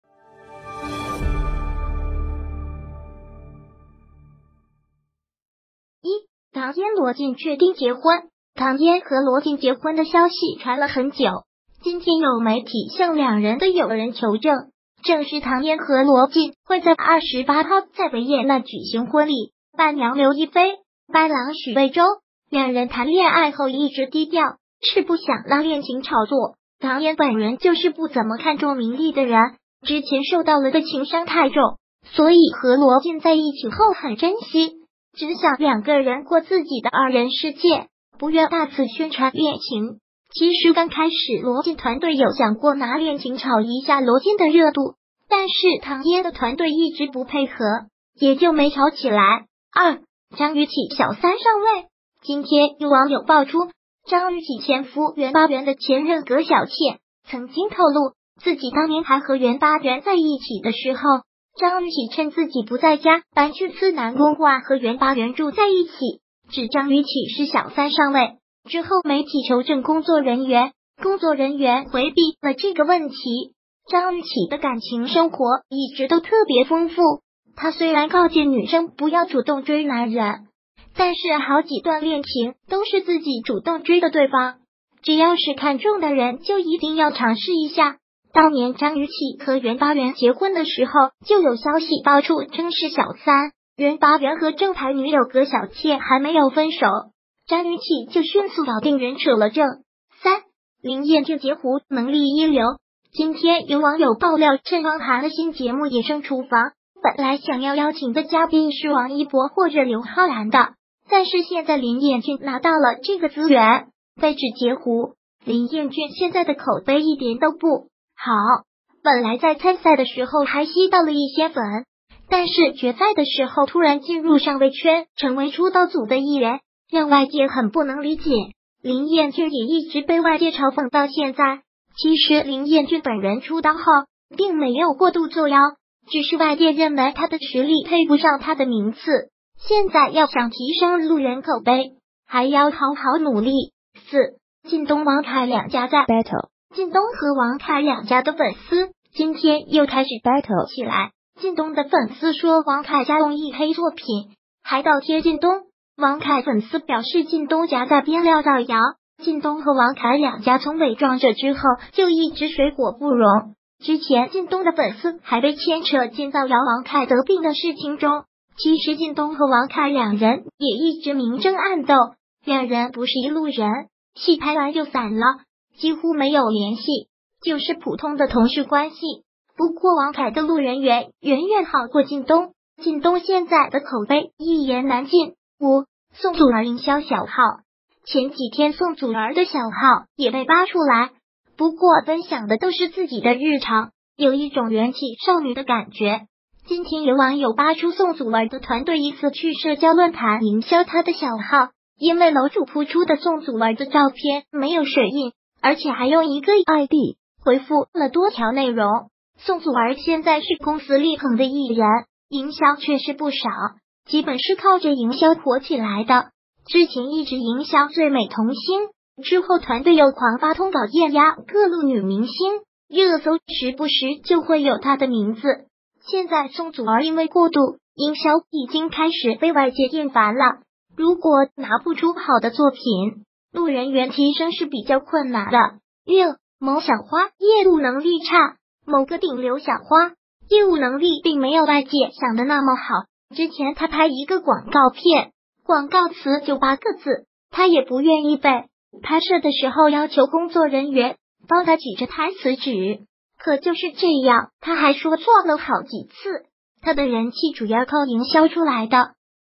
张雨绮小三上位？宋祖儿小号被扒？唐嫣罗晋月底结婚？林彦俊截胡？小花业务能力差？小扒问答
1.唐嫣罗晋确定结婚？唐嫣和罗晋结婚的消息传了很久，今天有媒体向两人的友人求证，证实唐嫣和罗晋会在28号在维也纳举行婚礼，伴娘刘亦菲，伴郎许魏洲。两人谈恋爱后一直低调，是不想拿恋情炒作，唐嫣本人就是不怎么看重名利的人，之前受到的情伤太重，所以和罗晋在一起后很珍惜，只想两个人过自己的二人世界，不愿大肆宣传恋情。其实刚开始罗晋团队有想过拿恋情炒一下罗晋的热度，但是唐嫣的团队一直不配合，也就没炒起来。2.张雨绮小三上位？今天有网友曝出，张雨绮前夫袁巴元的前任葛晓倩曾经透露自己当年还和袁巴元在一起的时候，张雨绮趁自己不在家搬去思南公馆和袁巴元住在一起，指张雨绮是小三上位，之后媒体求证工作人员，工作人员回避了这个问题。张雨绮的感情生活一直都特别丰富，她虽然告诫女生不要主动追男人，但是好几段恋情都是自己主动追的对方，只要是看中的人就一定要尝试一下，当年张雨绮和袁巴元结婚的时候就有消息曝出称是小三，袁巴元和正牌女友葛晓倩还没有分手，张雨绮就迅速搞定袁扯了证。3.林彦俊截胡能力一流？今天有网友爆料称汪涵的新节目《野生厨房》本来想要邀请的嘉宾是王一博或者刘昊然的，但是现在林彦俊拿到了这个资源，被指截胡。